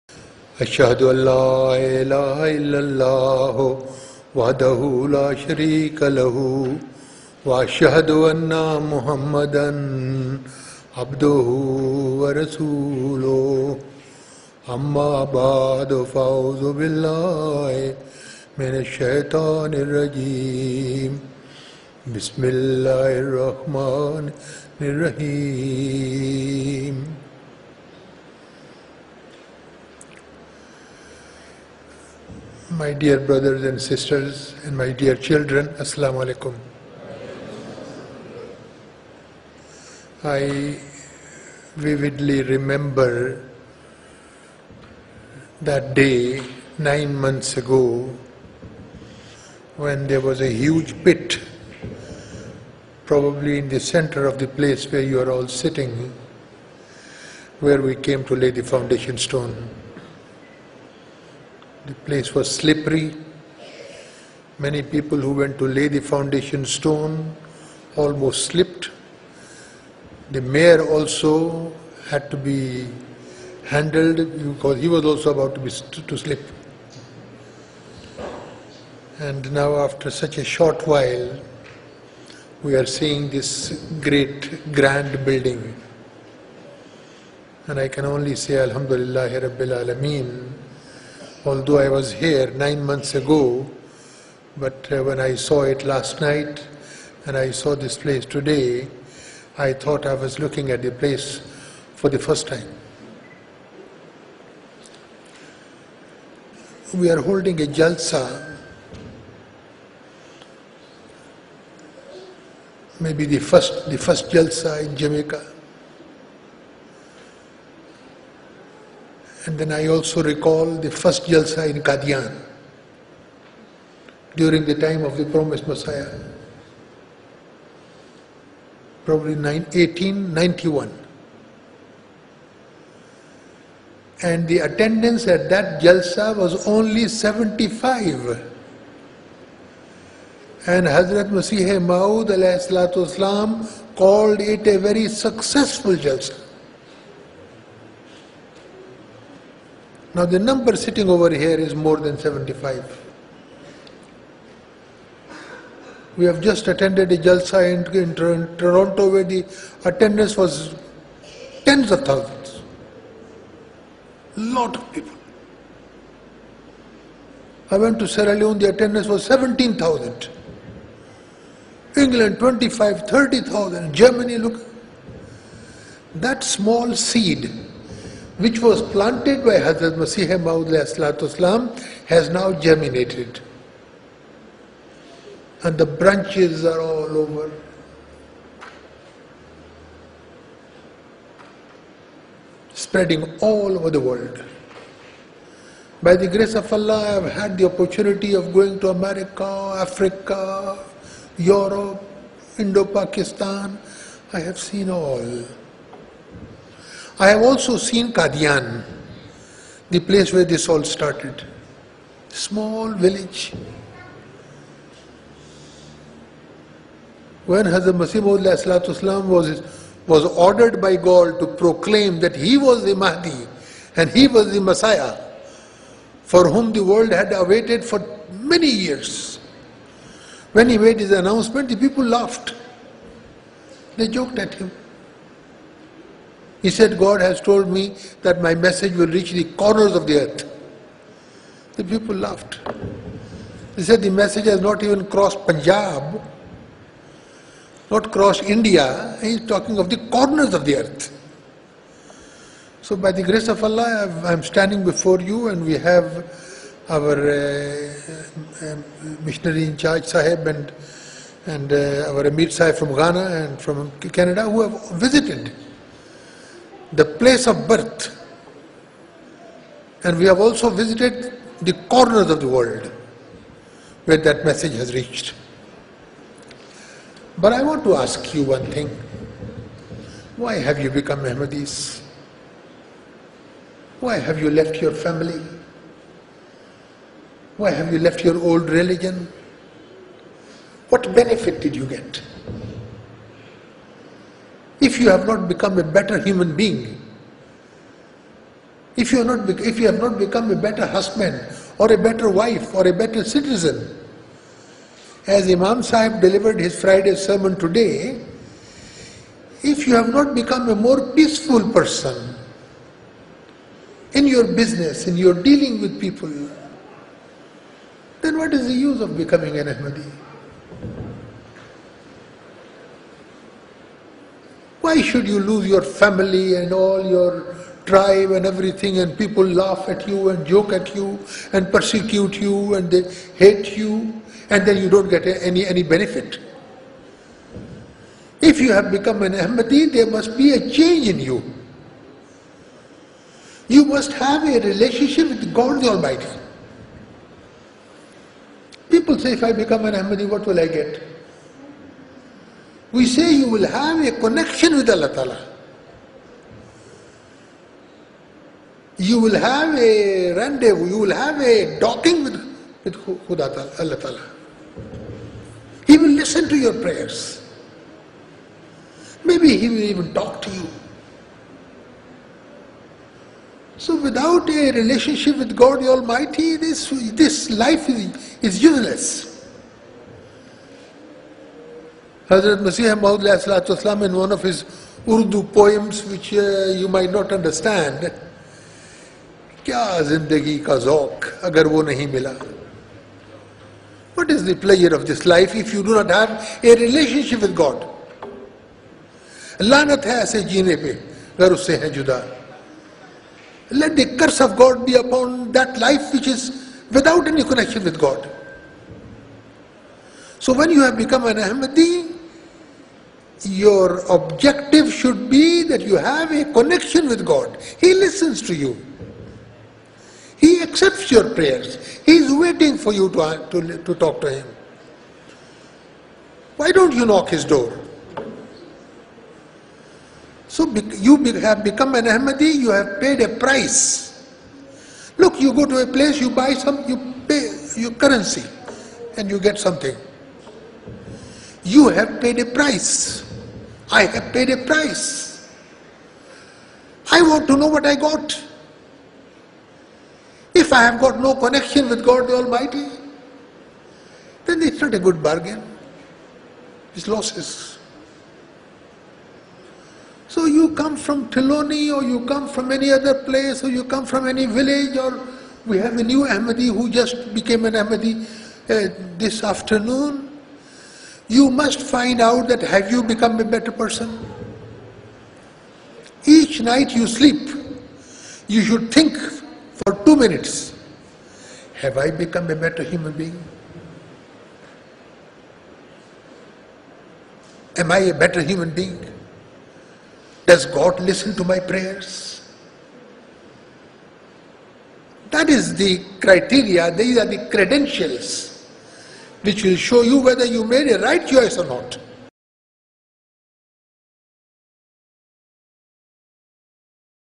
Why become an Ahmadi Muslim?
This is a small clip from Maulana Mubarak Nazeer's speech at the first Ahmadiyya Jalsa Salana in Jamaica in July, 2011. May Allah enable us to spread the message of Islam.